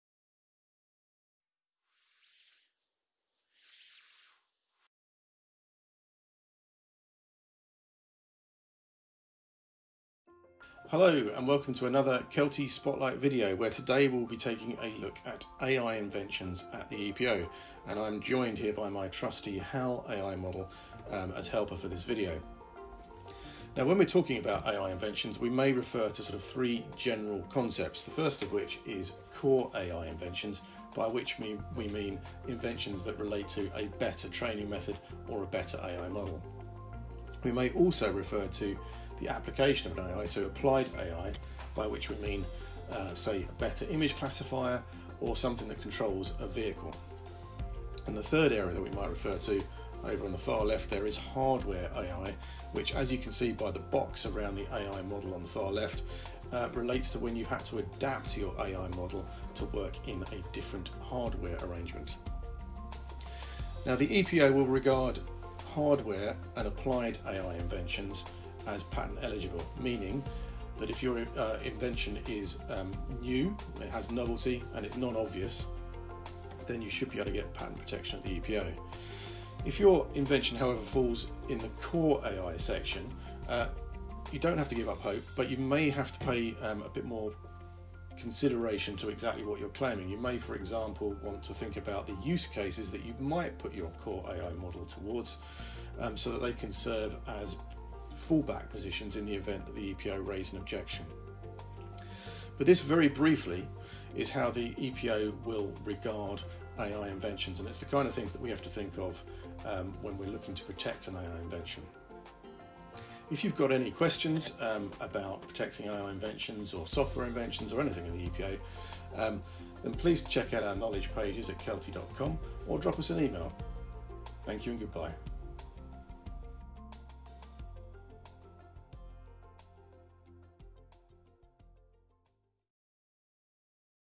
Spotlight Series: Protecting AI inventions at the European Patent Office (EPO)
In this latest instalment of Keltie's Spotlight Series, Mark Richardson provides an insight into the different types of AI inventions that are patentable at the EPO.